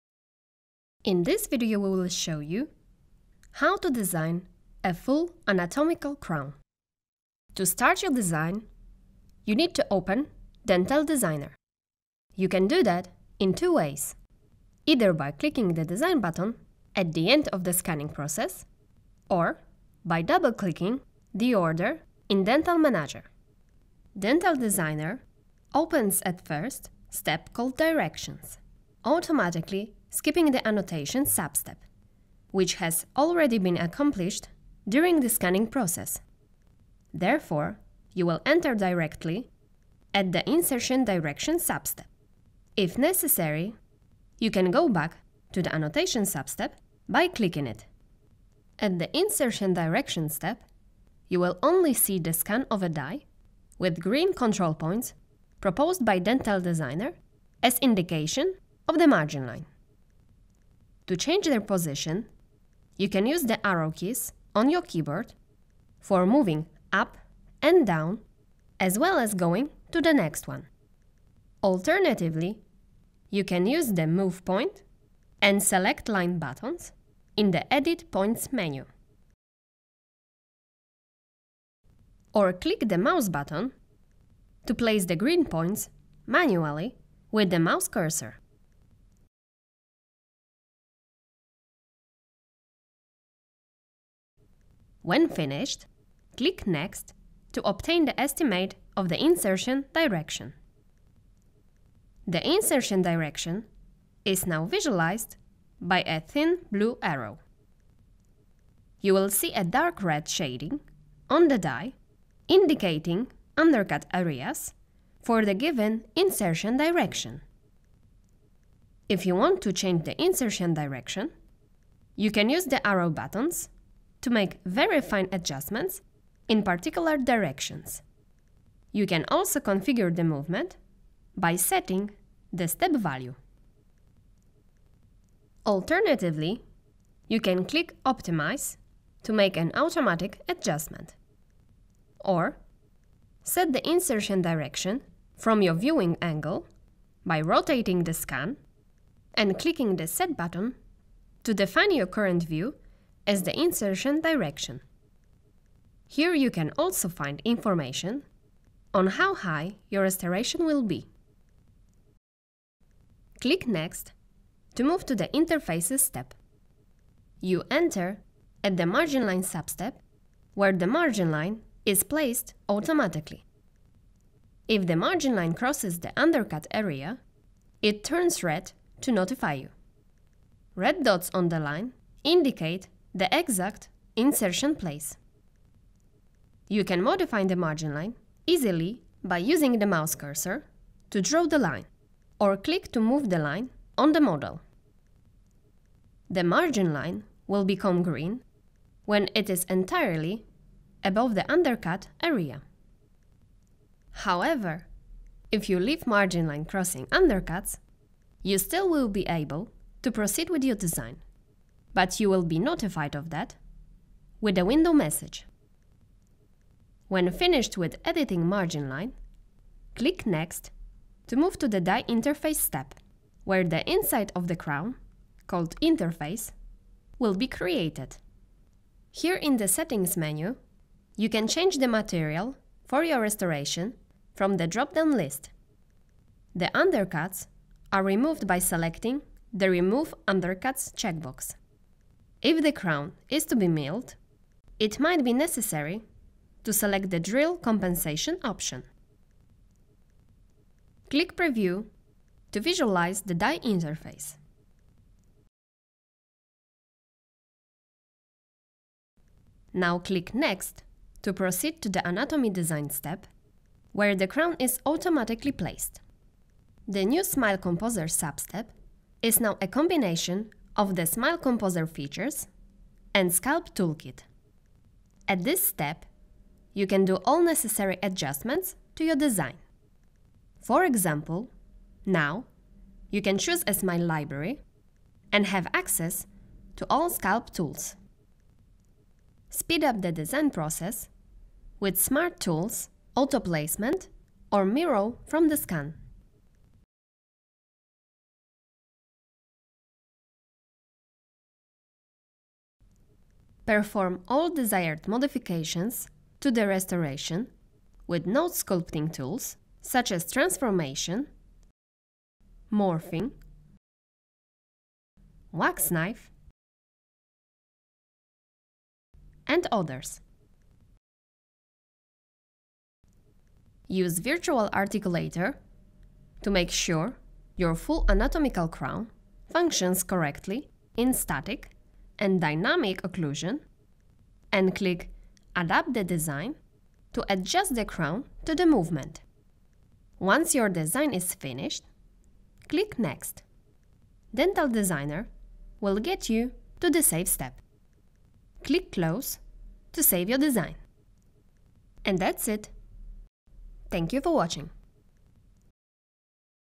3Shape Dental System - Full Anatomical Crown Design
In this video, we will show you how to design a full anatomical crown. You will be able to understand the workflow, tools and settings used in the design process.